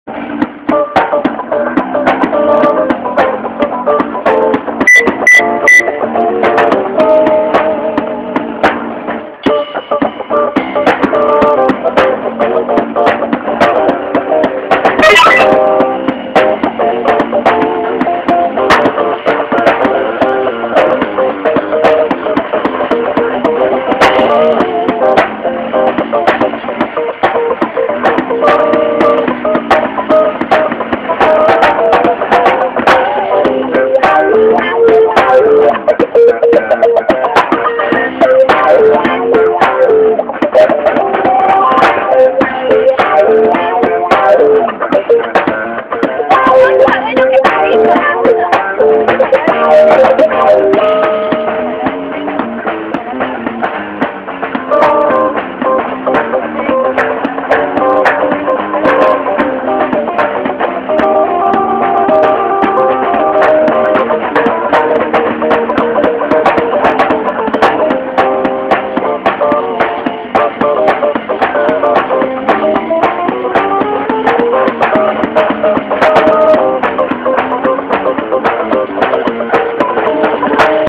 Funk en la plaza
Plaza 9 de julio, Posadas, Argentina. Una mañana cualquiera te puede sorprender con un poco de buena onda en medio de la rutina diaria :)